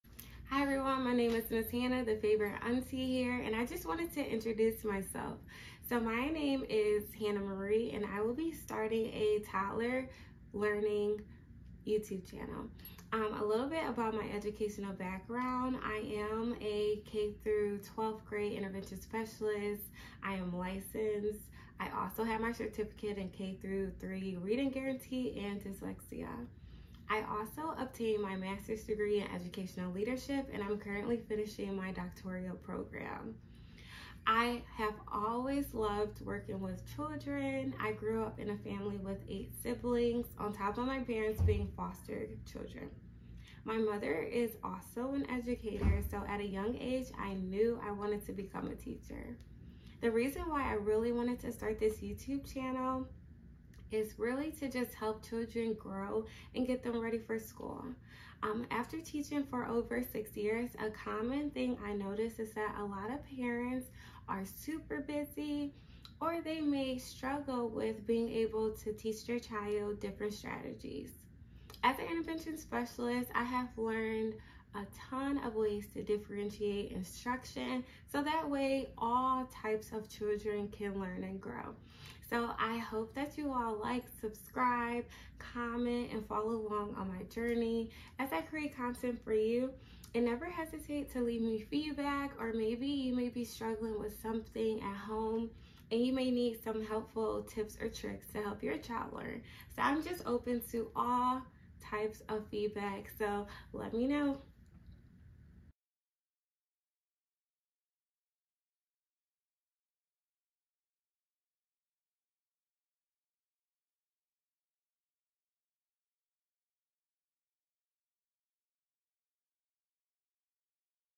Meet Ms. Hannah! Toddler Learning- Educational Videos for Toddlers-Teacher Approved Screen Time!
Meet Ms. Hannah! Ms. Hannah is a passionate and highly qualified teacher with a Bachelor’s degree in Early Childhood Education and a Master’s degree in Educational Leadership. Ms. Hannah is a Licensed k-12 Intervention Specialist with a k-3rd grade Reading Guarantee Endorsement and Dyslexia Certificate. She has a unique talent for engaging with toddlers and babies and has found an innovative way to use her skills to benefit young children around the world.

Ms. Hannah creates interactive videos that are specifically designed to help prepare kids for preschool. These videos are filled with fun and educational content that helps children learn important concepts such as colors, shapes, letters, and numbers. They also help to develop important social skills such as sharing, taking turns, and following directions. Her videos are full of bright colors, fun music, and engaging activities that keep kids entertained while they learn.

In a world where technology is becoming more and more prevalent, Ms. Hannah has found a way to use it to benefit young children positively. So come Learn and Grow with Ms. Hannah. Teacher Approved Screen Time!